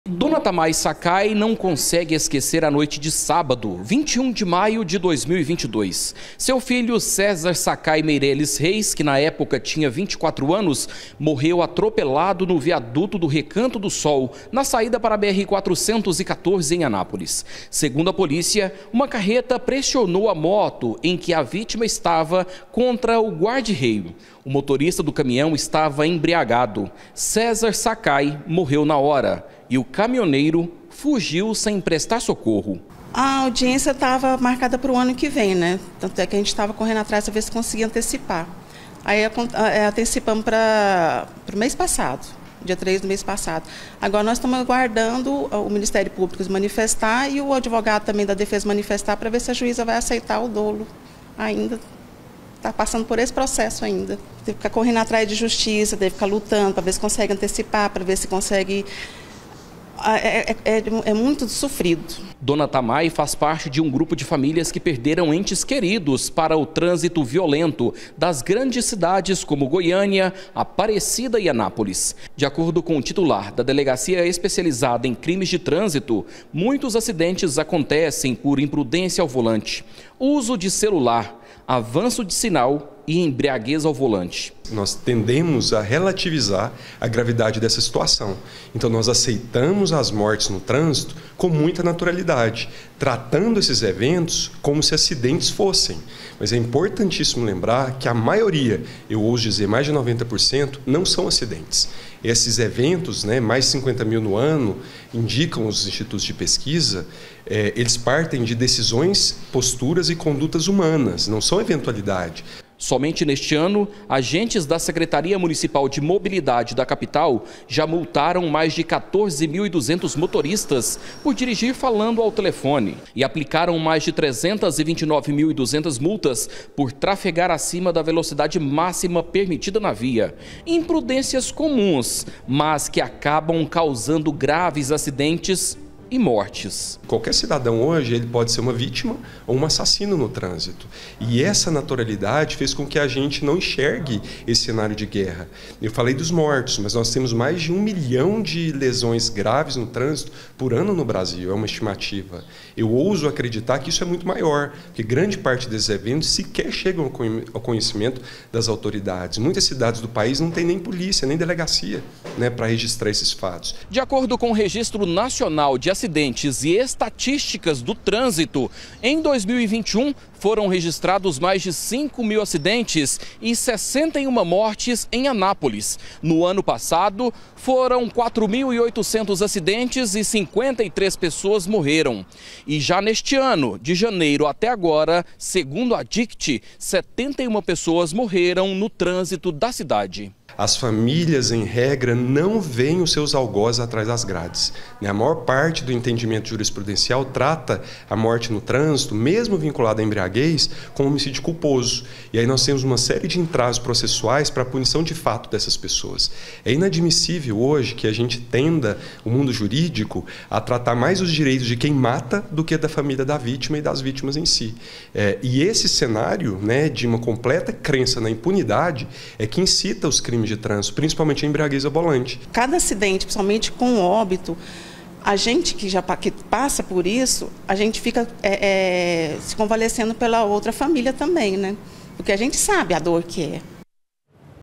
ACIDENTES FATAIS: LEGISLAÇÃO FAVORECE IMPUNIDADE NO TRÂNSITO
ACIDENTES DE TRÂNSITO COM VÍTIMAS FATAIS QUASE SEMPRE TEM A IMPRUDÊNCIA COMO FATOR CAUSADOR. AINDA ASSIM CASOS GRAVES COMO MORTES CAUSADAS POR MOTORISTAS BÊBADOS AINDA PERMANECEM IMPUNES.